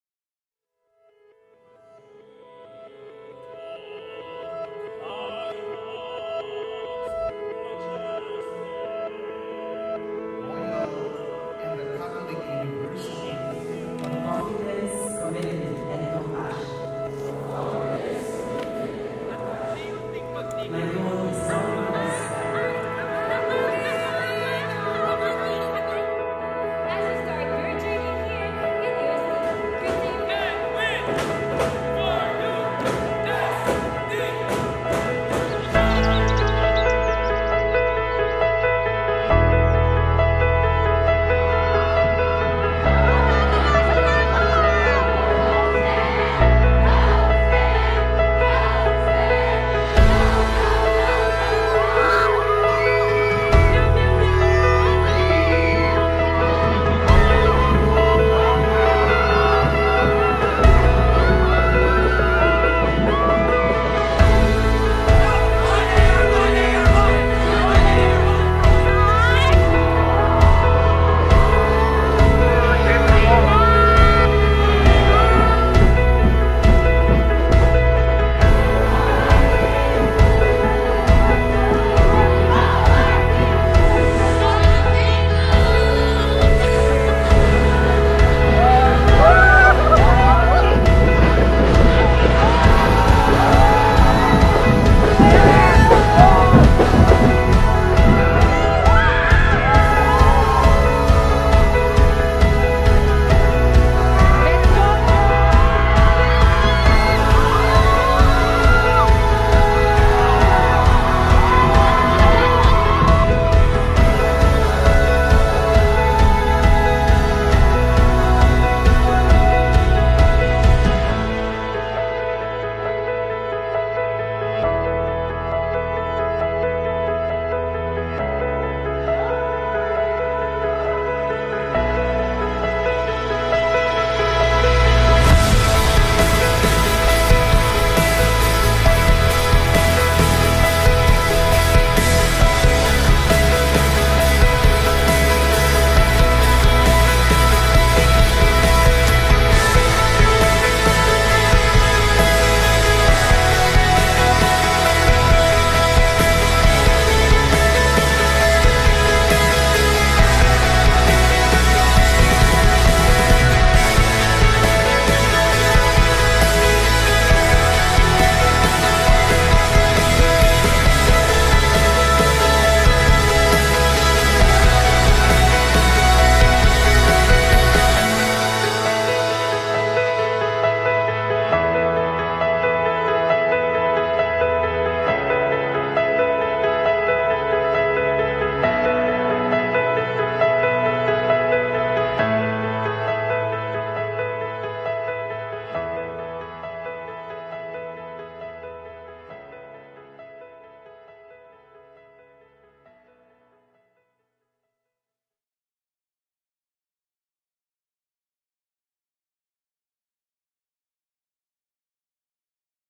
Thomasian Freshman Week 2019
Sa pagharap sa panibagong yugto, sa pagtahak ng panibagong landas, ay kalakip ang mga bagong ala-ala, kwento, at saya. Nagsimula na ang iyong paglalakbay, ating sariwain ang kaganapan noong Freshman Week.

Maligayang pagdating, mga Tomasino!

Videographers:
Gemiel Bongcales
Anne Cabanza
Alexa Catindig
James Gomez
Anthony Tria
Julius Villavieja
Troy Quinan

Editor:
Anthony Tria